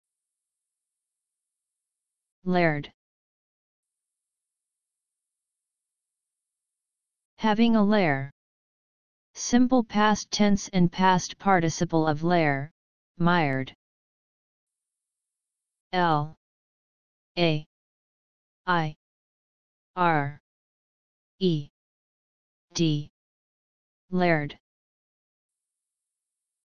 what is the meaning of laired.
what laired means:
Having a lair. simple past tense and past participle of lair; mired.